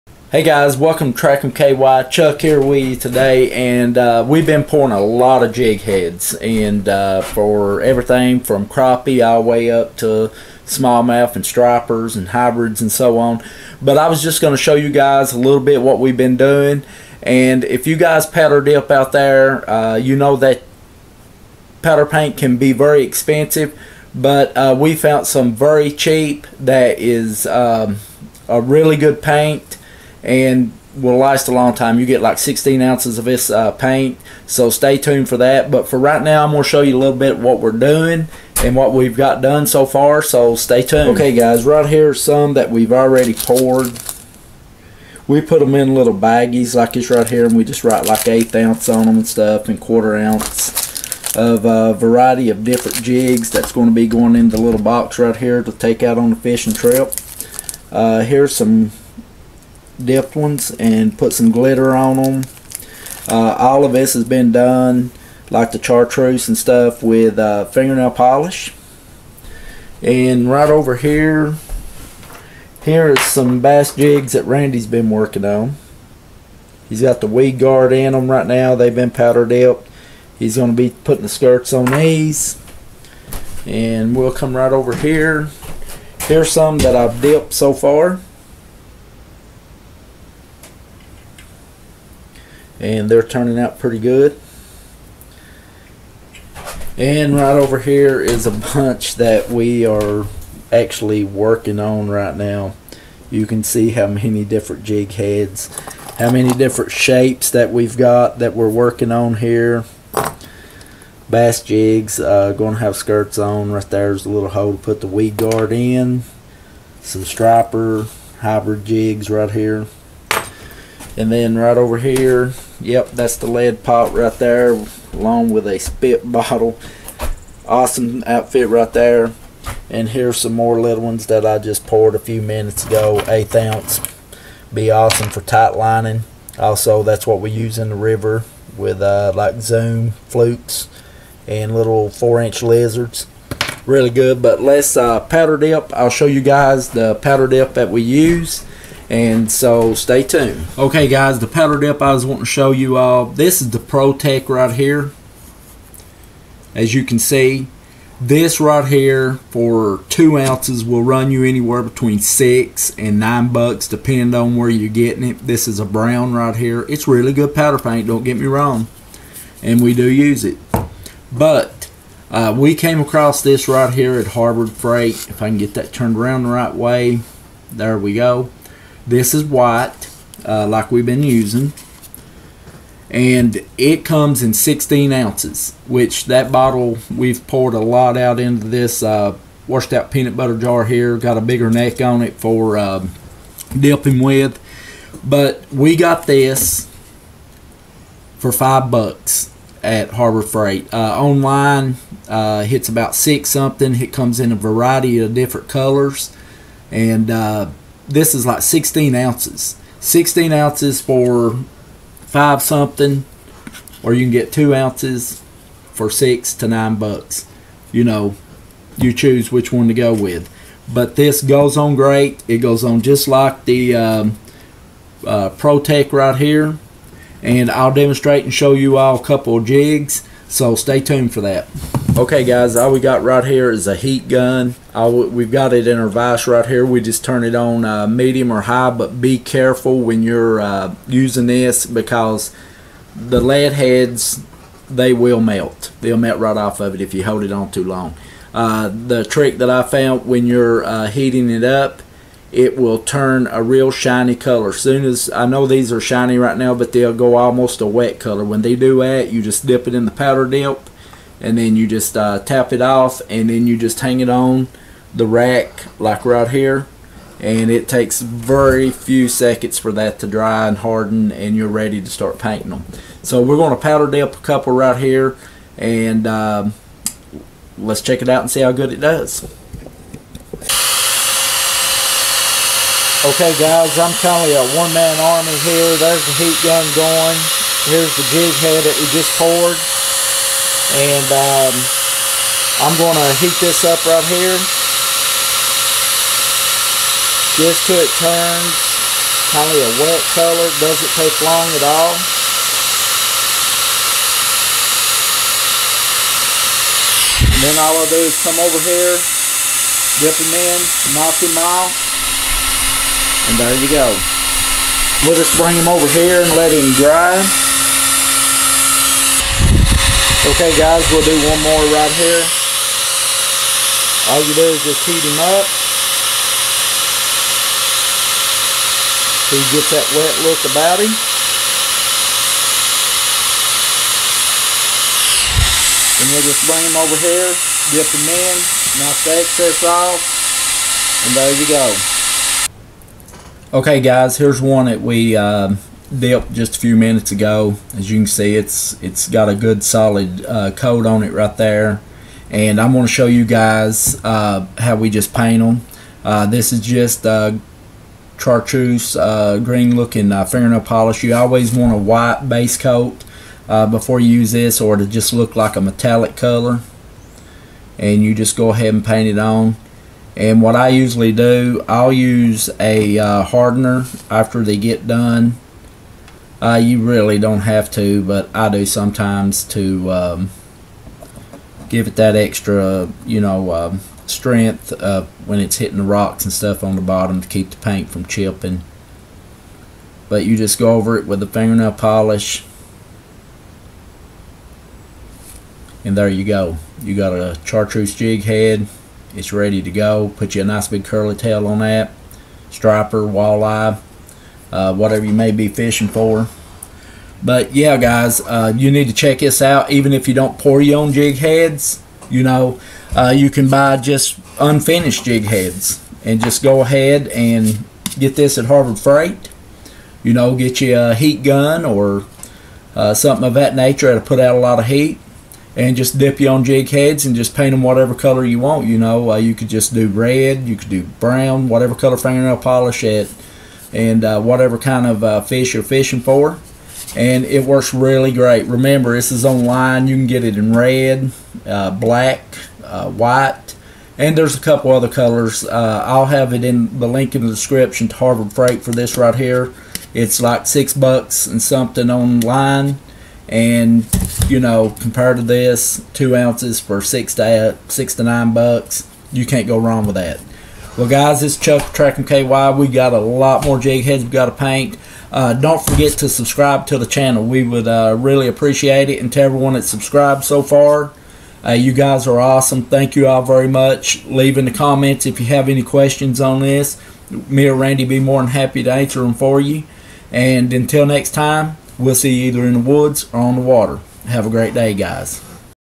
Powder Coat jigheads Cheap with paint from Harbor Freight Review
Powder coat paint for dipping  jigs can be expensive with 2oz as high as $ 9.00 but at Harbor Freight you can get 16oz for little over $6.00.